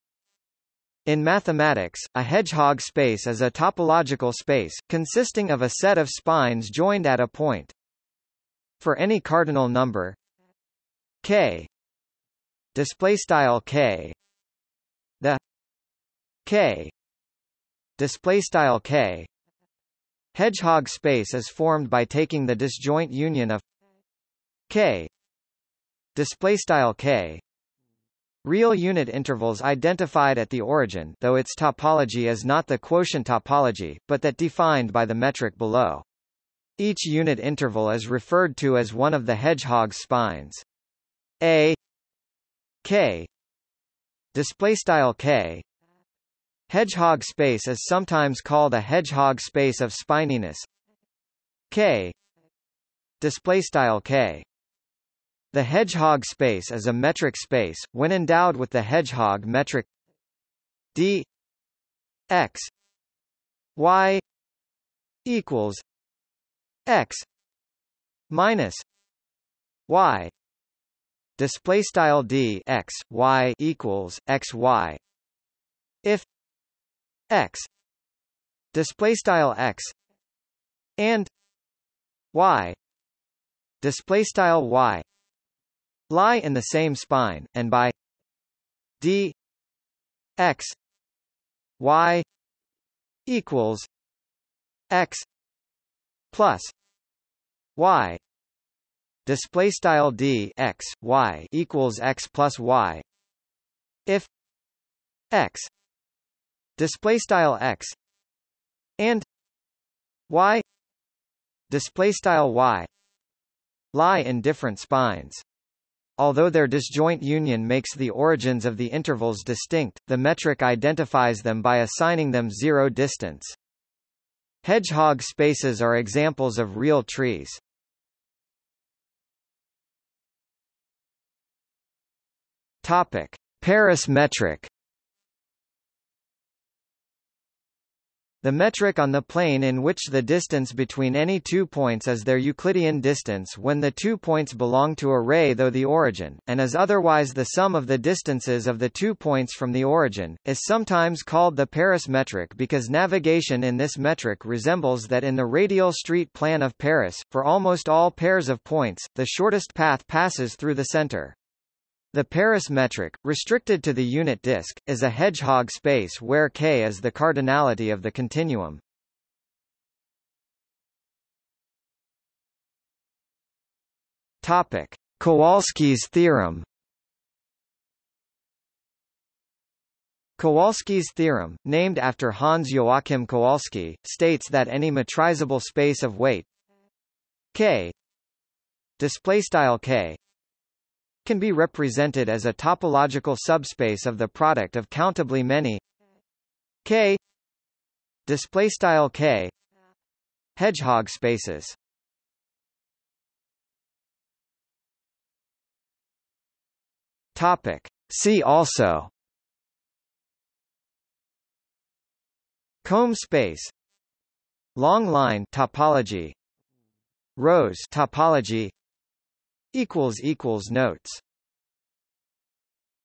Hedgehog space | Wikipedia audio article
00:02:09 1 Paris metric
00:02:50 2 Kowalsky's theorem
00:03:23 3 See also

- Socrates

SUMMARY
In mathematics, a hedgehog space is a topological space, consisting of a set of spines joined at a point.
For any cardinal number

        K

    {\displaystyle K}
  , the

        K

    {\displaystyle K}
  -hedgehog space is formed by taking the disjoint union of

        K

    {\displaystyle K}
   real unit intervals identified at the origin (though its topology is not the quotient topology, but that defined by the metric below). Each unit interval is referred to as one of the hedgehog's spines. A

        K

    {\displaystyle K}
  -hedgehog space is sometimes called a hedgehog space of spininess

        K

    {\displaystyle K}
The hedgehog space is a metric space, when endowed with the hedgehog metric

        x
        y

    {\displaystyle d(x,y)=|x-y|}
   if

        x

    {\displaystyle x}
   and

        y

    {\displaystyle y}
   lie in the same spine, and by

        d
        x
        y
        x
        y

    {\displaystyle d(x,y)=x+y}
   if

        x

    {\displaystyle x}
   and

        y

    {\displaystyle y}
   lie in different spines. Although their disjoint union makes the origins of the intervals distinct, the metric identifies them by assigning them 0 distance.
Hedgehog spaces are examples of real trees.